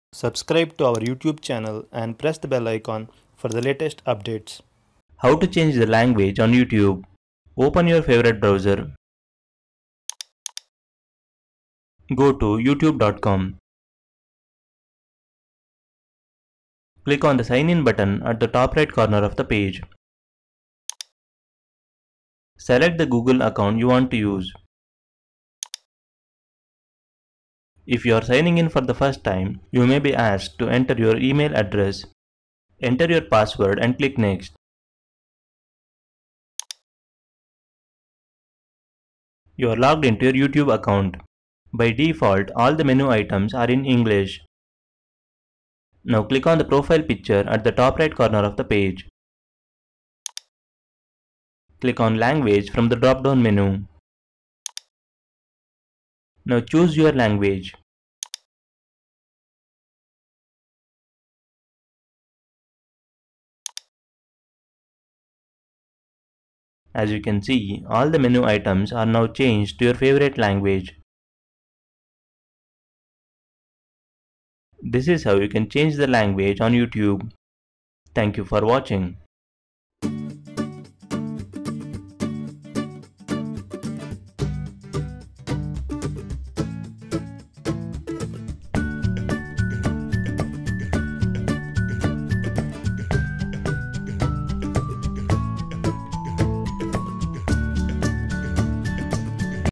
How to Change the Language on Youtube
0:00 Subscribe to Our Youtube Channel and Press the Bell Icon for Latest Updates
0:05 How to Change the Language on Youtube
0:07 Open Your Favourite Browser.
0:11 Go to Youtube.com
0:16 Click on the “Sign in” Button at the Top Right Corner of the Page.
0:22 Select the Google Account You Want to Use.
0:27 If You are Signing in For the First Time, You May be asked to Enter Your Email Address.
0:32 Enter Your Password and Click “Next”.
0:39 You are Logged into Your Youtube Account.
0:41 By Default All the Menu Items are in English.
0:45 Now, Click on the “Profile Picture” at the Top Right Corner of the Page.
0:51 Click on “Language” from the Drop Down Menu.
0:56 Now, Select Your Preferred Language.
1:06 As You Can See, All the Menu Items are Now Changed to Your Preferred Language.
1:14 This is How You Can Change the Language on Youtube
1:18 Thank You for Watching.

Translations